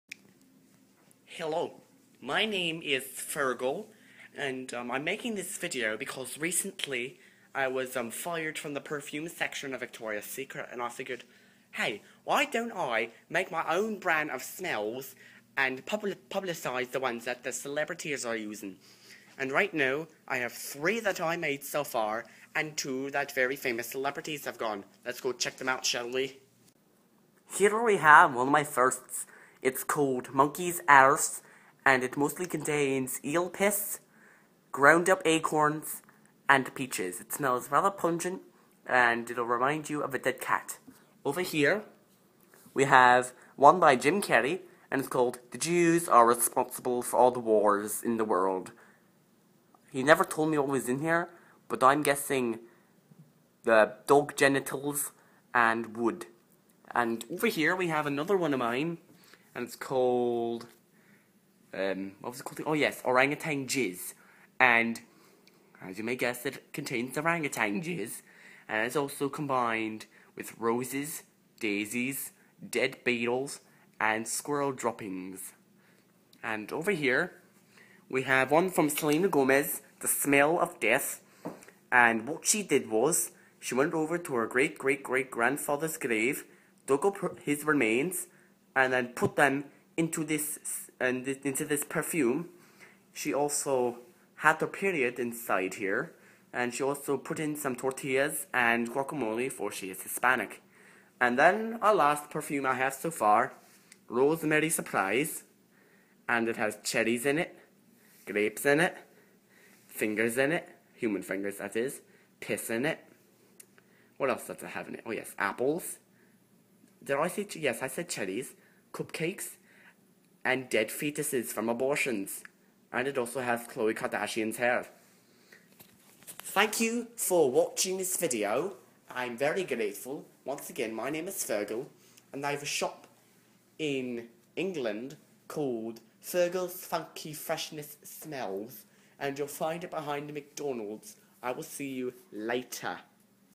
Fearghal's Perfumes: part 1
Fearghal shows off his newest perfumes!